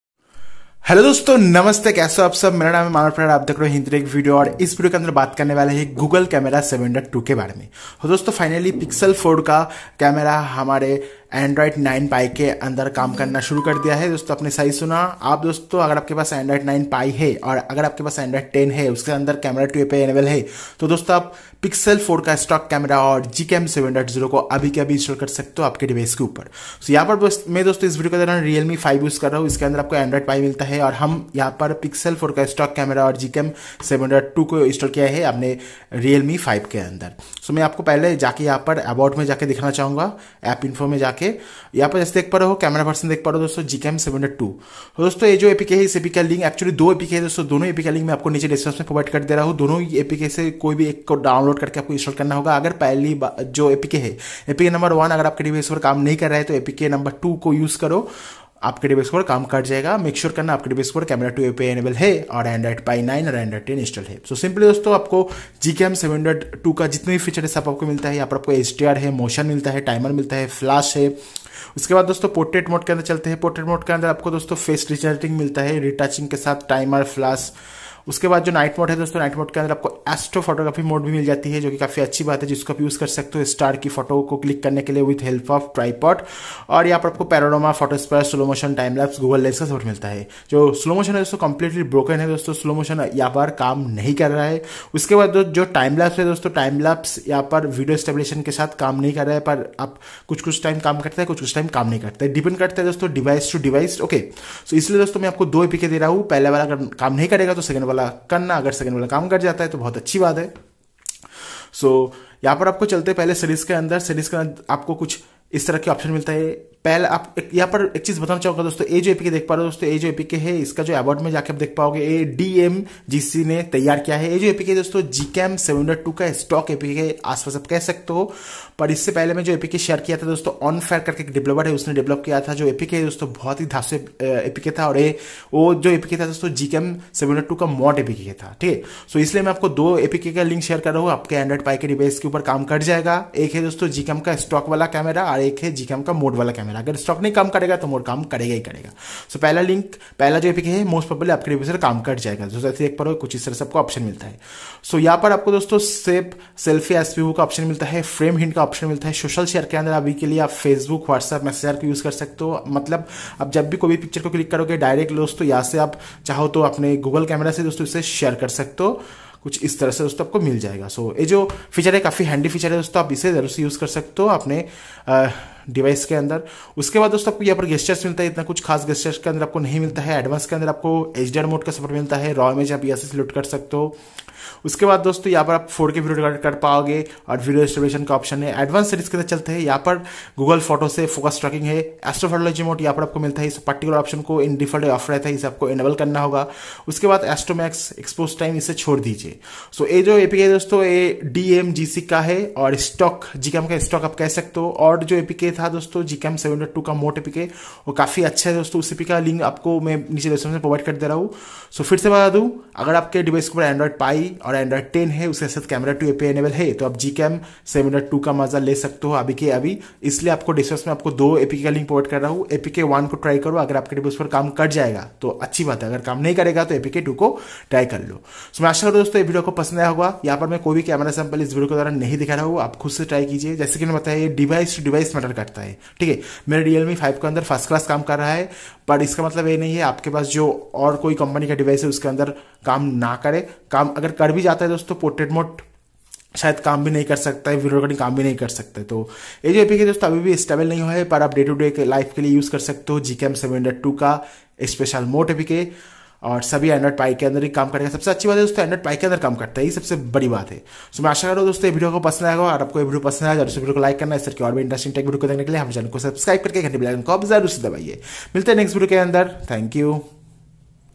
Google Camera 7.2 apk for Android 9 Pie ft. Realme 5 | Astro Mode | Night Slight | Portrait
🔥 Gcam 7.0 V2 Apk on Any Android ft. Redmi 4a